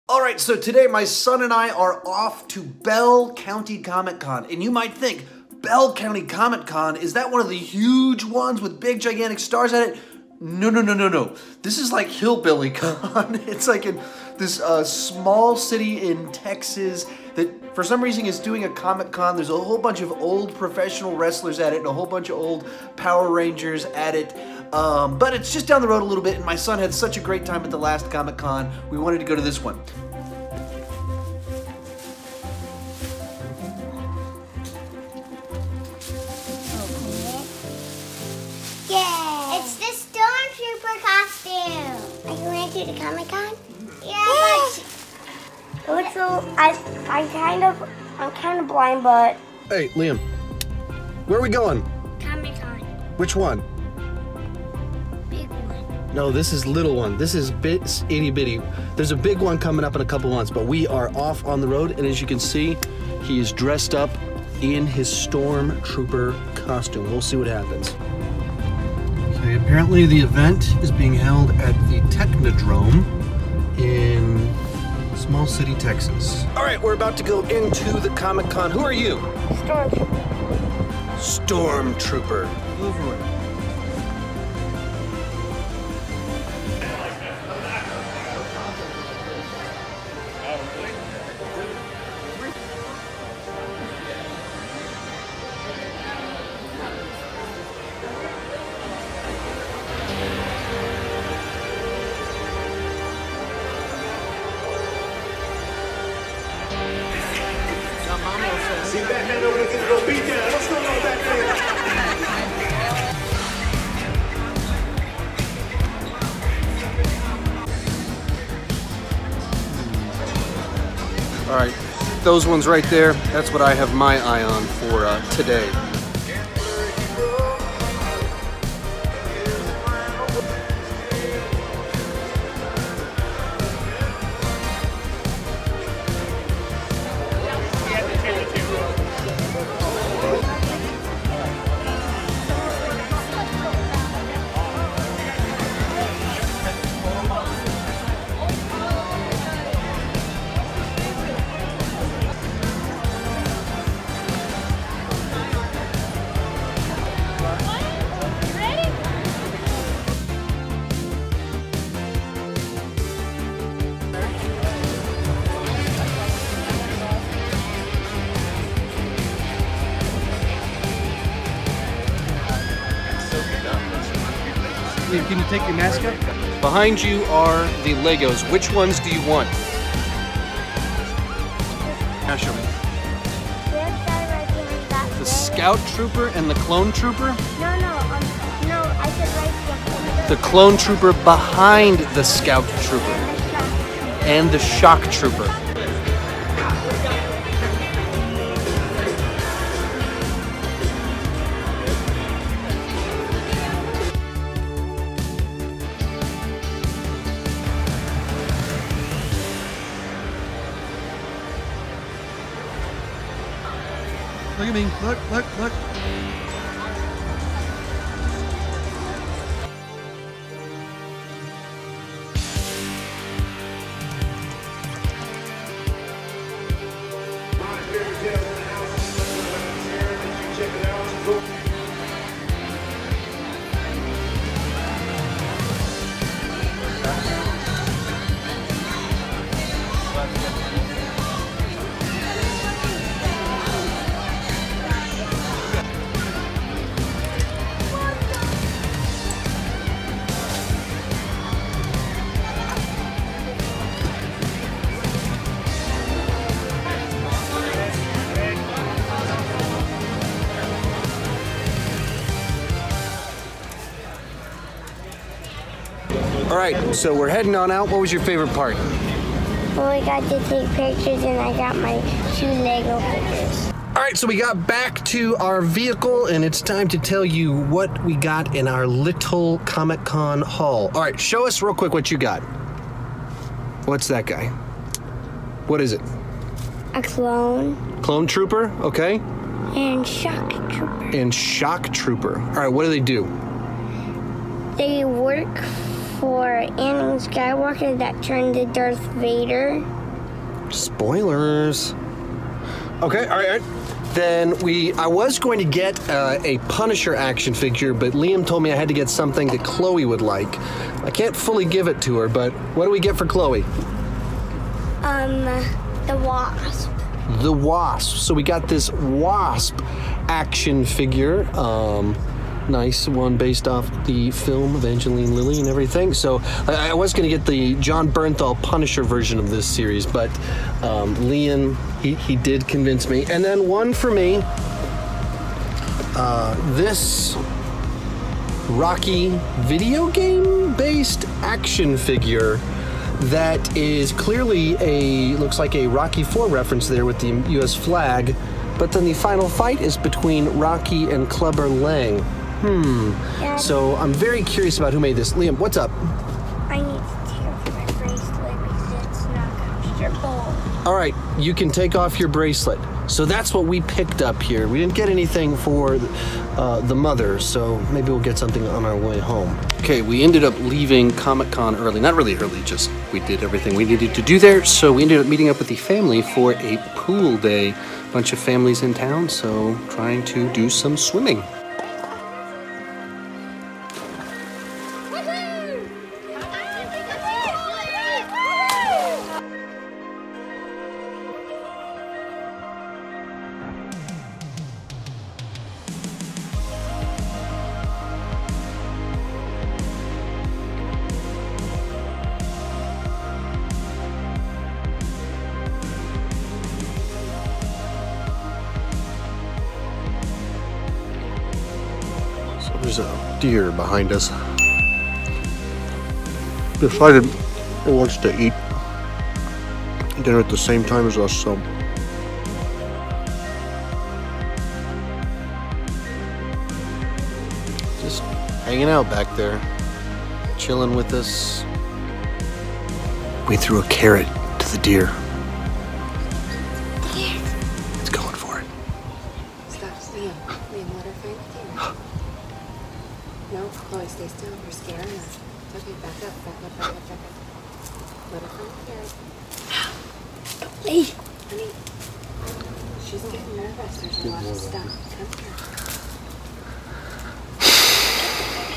VLOG #17 | Bell County Comic Con & Pool Day!
I take my son to Bell County Comic 2018 and then go swimming with some family.